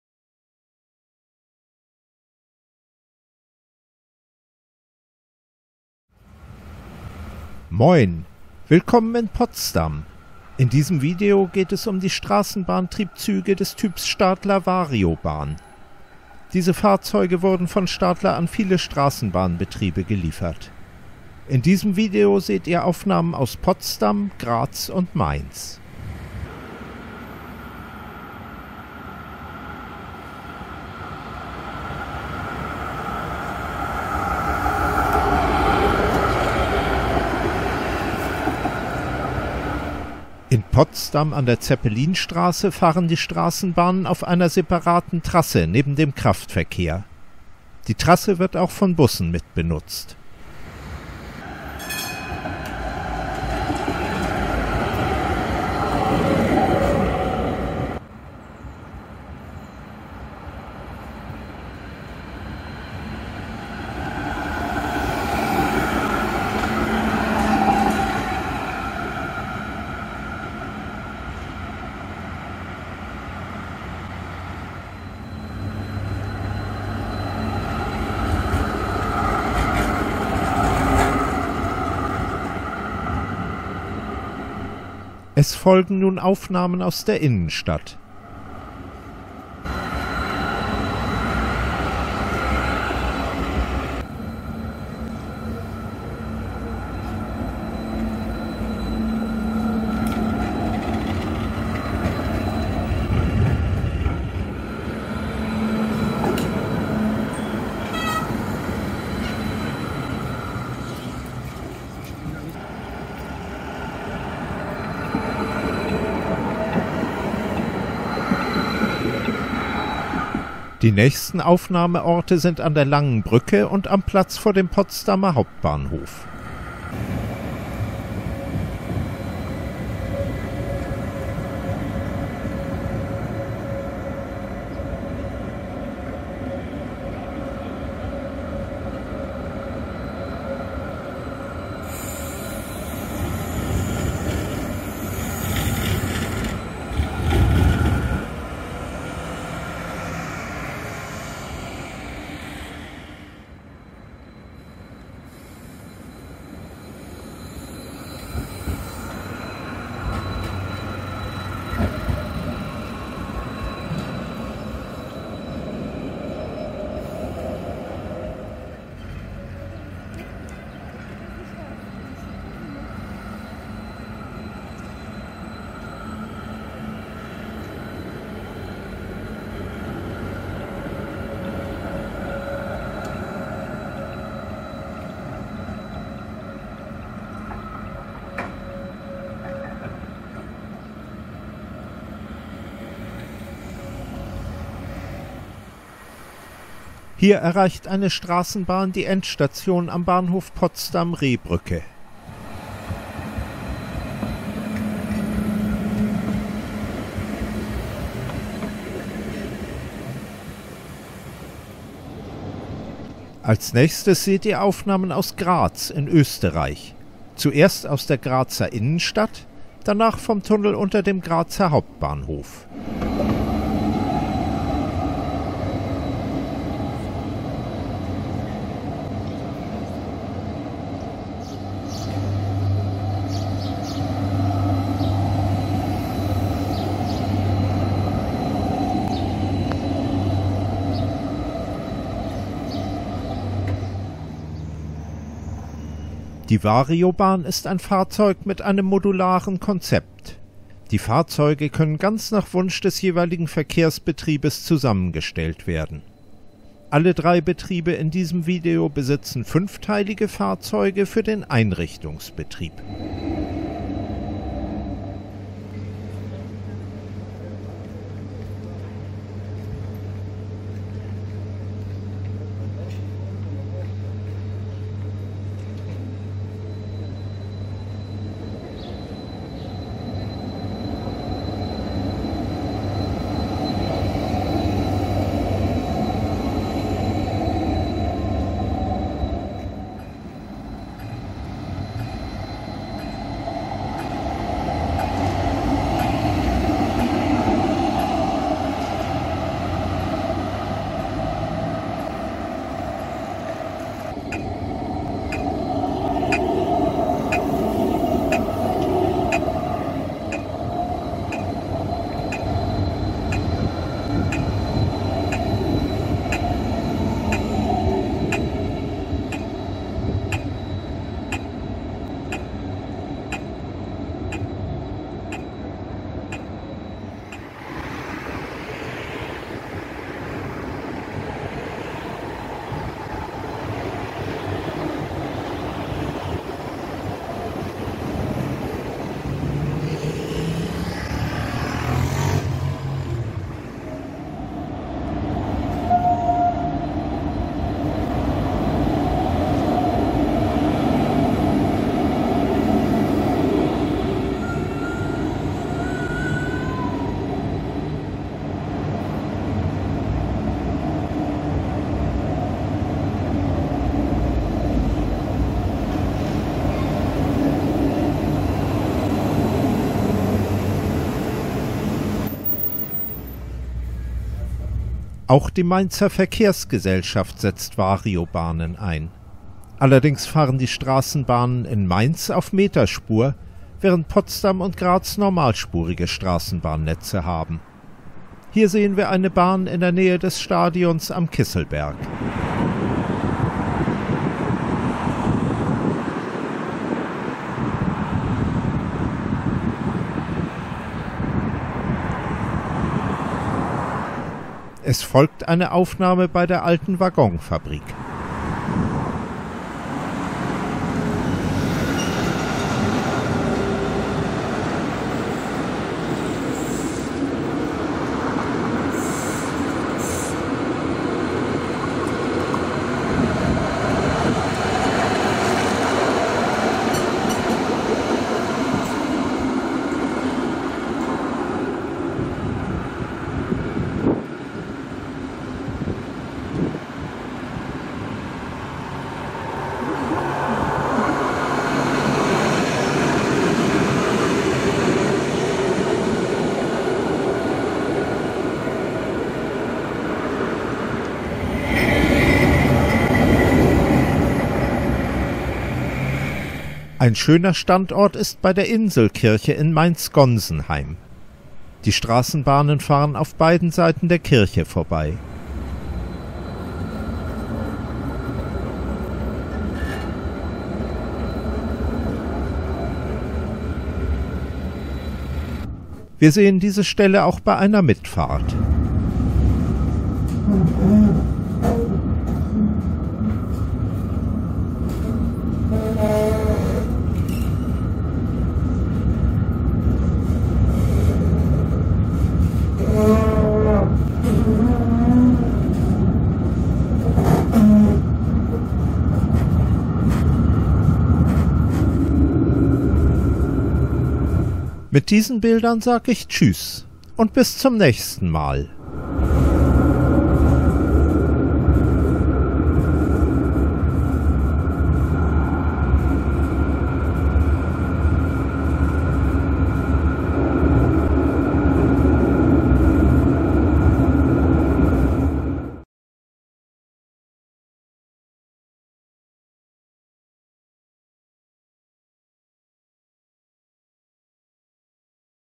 Straßenbahnen – Trams: Die Niederflur-Triebzüge Stadler Variobahn in Potsdam, Graz und Mainz
English subtitles available, English description below – Die Straßenbahn-Triebzüge des Typs Variobahn wurden von Stadler an viele Straßenbahnbetriebe geliefert. Dieses Video zeigt Aufnahmen aus Potsdam, Graz und Mainz.
Die Variobahn ist ein Fahrzeug mit einem modularen Konzept. Die Fahrzeuge können ganz nach Wunsch des jeweiligen Verkehrsbetriebes zusammengestellt werden. Alle drei Betriebe in diesem Video besitzen fünfteilige Fahrzeuge für den Einrichtungsbetrieb. Potsdam und Graz haben normalspurige Straßenbahnnetze. In Mainz fahren die Straßenbahnen auf Meterspur.
Graz besitzt 45 Fahrzeuge, Potsdam 18 und Mainz 19 Fahrzeuge. Die Wagen in Graz haben eine Länge von 27,5 m, in Potsdam 29,6 m und in Mainz 30,0 m. Die Länge und andere Abmessungen hängen von den Gegebenheiten des jeweiligen Straßenbahnnetzes ab, u. a. vom Lichtraumprofil und vom Kurvenradius.
In allen 3 Städten sind nicht nur Variobahnen, sondern auch andere Typen von Straßenbahnfahrzeugen im Einsatz.

Dieses Video entstand in Zusammenarbeit mit dem Inhaber des YouTube-Kanals garethtrooper (@garethtrooper), der die Videos aus Graz zur Verfügung gestellt hat.

Aufnahmeorte / Locations
00:00 Potsdam, Zeppelinstraße
01:16 Potsdam, Innenstadt / city center
01:54 Potsdam, Lange Brücke
02:07 Potsdam, Hauptbahnhof / central station
03:16 Potsdam, nahe Bhf Rehbrücke / near Rehbrücke station
03:32 Graz, Innenstadt /city center
05:04 Graz, beim Hauptbahnhof / near central station
06:14 Mainz, Neustadt/Mombach
06:27 Mainz, Kisselberg
06:50 Mainz, Waggonfabrik
07:32 Mainz, Gonsenheim, Elbestraße/Inselkirche
07:55 Mainz, Mitfahrten / tram rides

The articulated trams of type Variobahn have been delivered to many tram companies by Stadler. In this video you can watch scenes filmed at Potsdam, Graz, and Mainz.
Variobahn is a tram vehicle based on a modular conception. The vehicles can be built just to the needs of the tram company ordering them. All 3 companies featured in this video use 5-section trams for single-directional operation. Potsdam and Graz have normal gauge tram networks (1435 mm). In Mainz trams run on meter gauge.
Graz has 45 Variobahn vehicles, Potsdam 18, and Mainz 19. The vehicles have slightly different lengths – Graz 27,5 m, Potsdam 29,6 m, and Mainz 30,0 m. Length and other dimensions depend on the situation in each tram network, like the radius of curves or the clearance.
In all 3 cities the tram companies do not only operate Variobahns, but also other types of tram vehicles.

This video was made in collaboration with the owner of YouTube channel garethtrooper (@garethtrooper) who provided the videos from Graz.